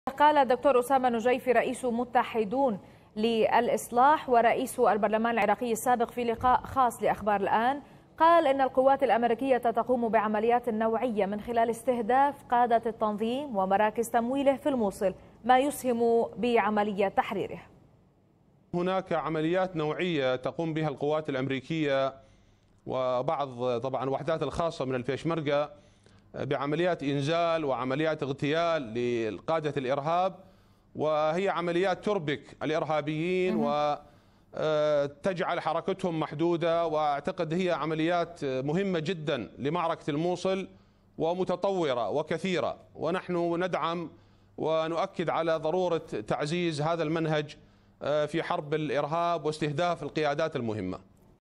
النجيفي: القوات الامريكية تقوم بعمليات نوعية لإغتيال قادة الإرهاب
قال الدكتور أسامة النجيفي رئيس متحدون للإصلاح ورئيس البرلمان العراقي السابق في لقاء خاص لأخبار الآن، قال إن القوات الأمريكية تقوم بعمليات نوعية..